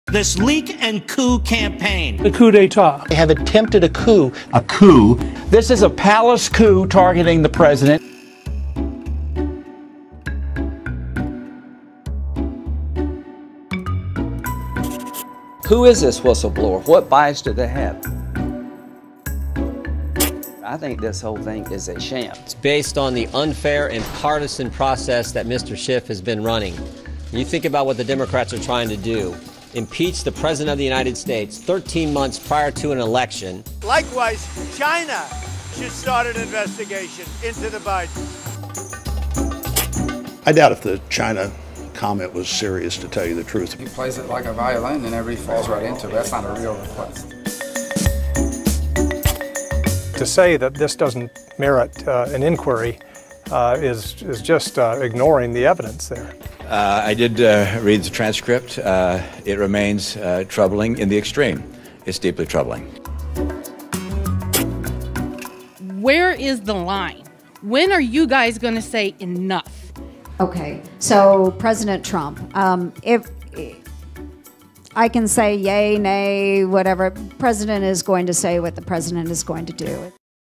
'Coup!' and other defences against Trump impeachment
Whether or not US President Donald Trump would get convicted in an impeachment trial could come down to the Republican majority in the Senate.

But what do Republican politicians and commentators think of impeachment and Trump's call with Ukraine?